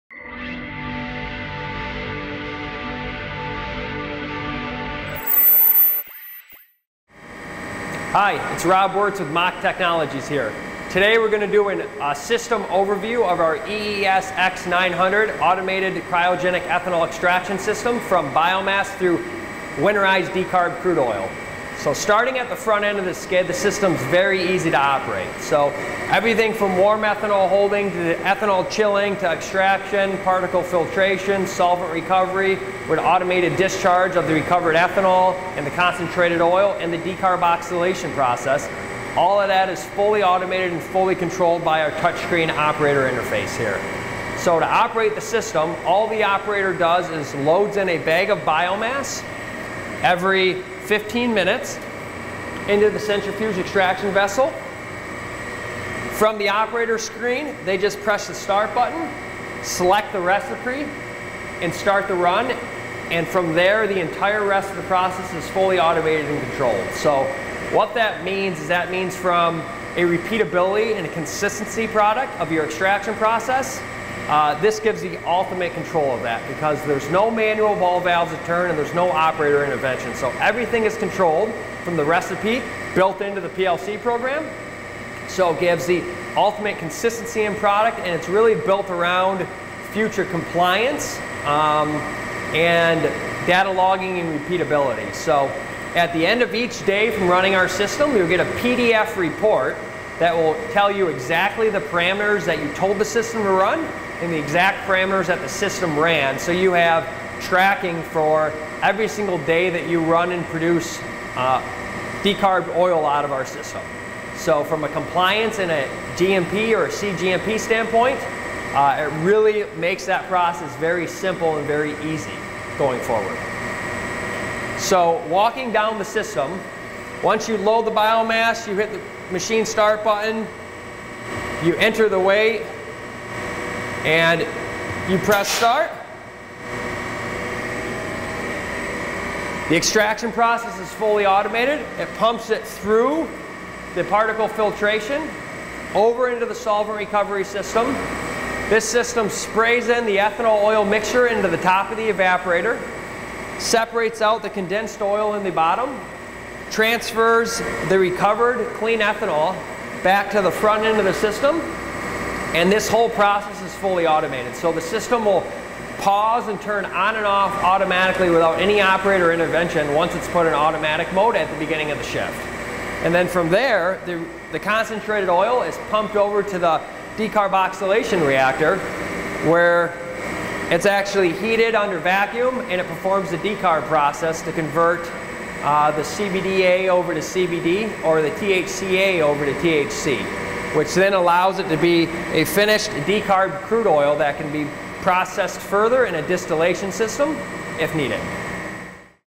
MACH Technologies EES X 900 Overview
This video will give you a great understanding of MACH Technologies EES-X-900 fully automated cryogenic ethanol extraction system! A completely recipe driven solution from biomass through either Winterized Decarbed Crude or Distillate.

We engineer and manufacture all of our automated extraction solutions for both Solvent and Solventless technologies out of Port Huron, MI and specialize in delivering turnkey extraction facilities that are 100% compliant and will have you operating with the best technology in the world!